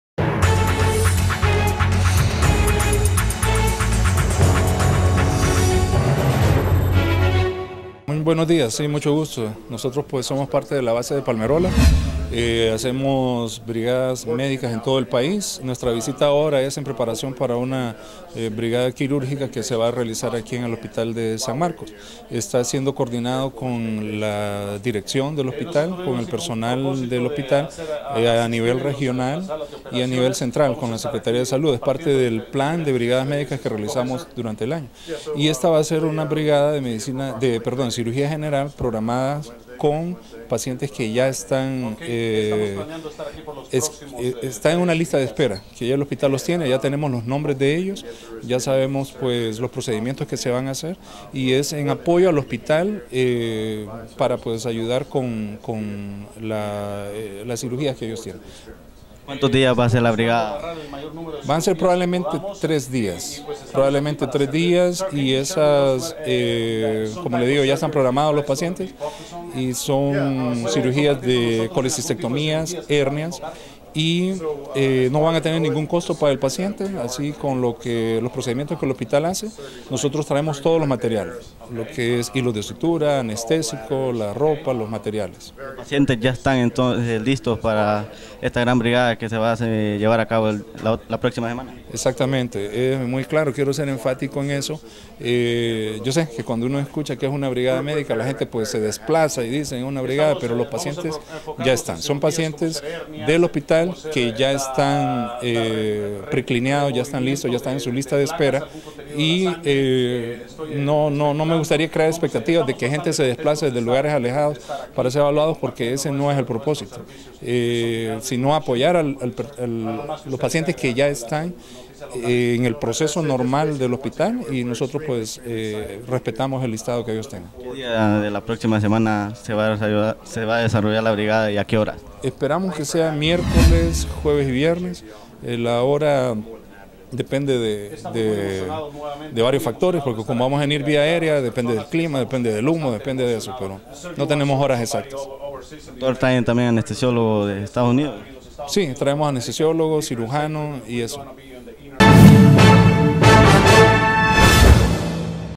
Brigada Médica en hospital de San Marcos
En los próximos días se realizará una brigada médica de cirugía general en el hospital departamental de San Marcos y el doctor Miguel Cuellar da más información sobre esta brigada que se realizará.

Canal de televisión de Ocotepeque, Honduras.